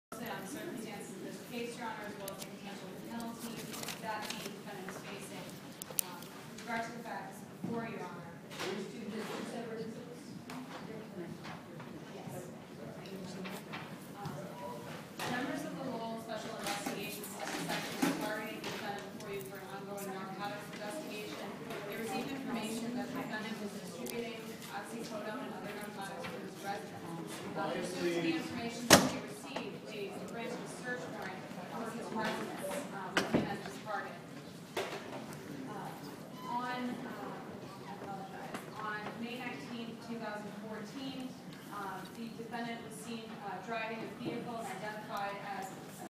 Wesley F. Philbrick, 28, of Lowell, held on $100,000 cash bail after Lowell police found a cache of
Wesley F. Philbrick, 28, of Lowell, held on $100,000 cash bail after Lowell police found a cache of weapons, drugs and $27,000 in cash in his 187 Mammoth Road home.@lowellsunnews